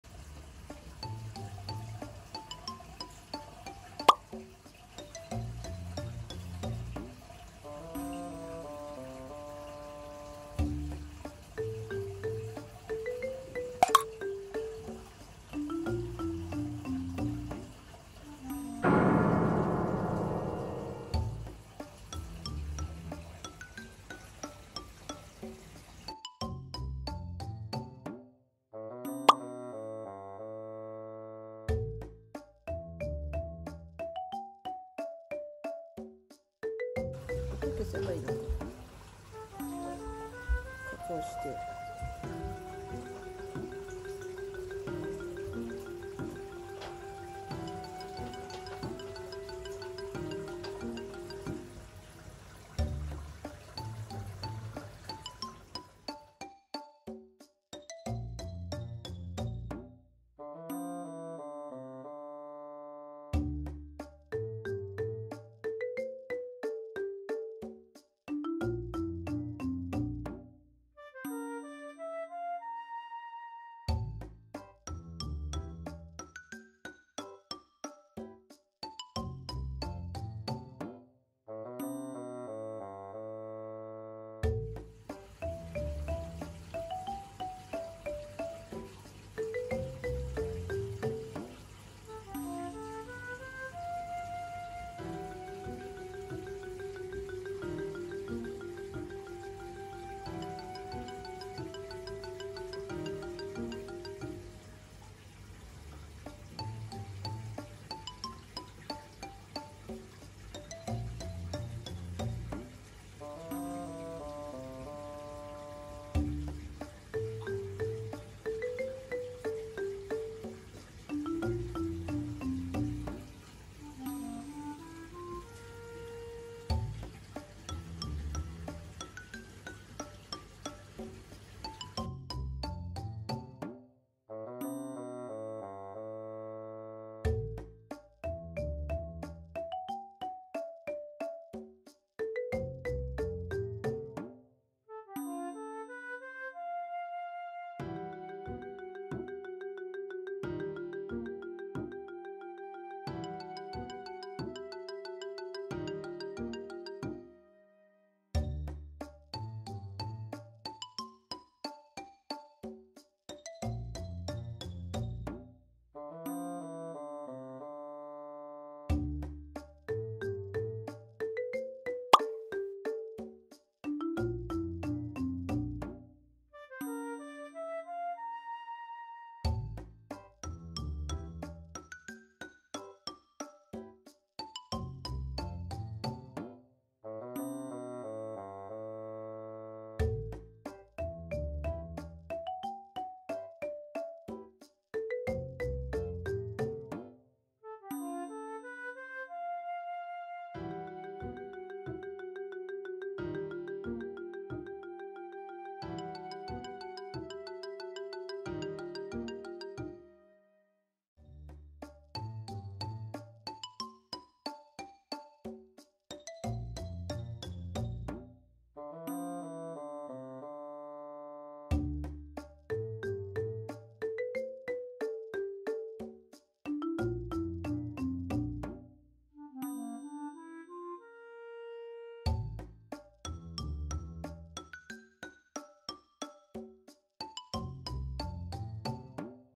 【海の魚とりガサガサ】透明の生き物フィーバー レアな魚ハナビラウオPsenes pellucidus
海の魚とりで採れた透明の生き物達です！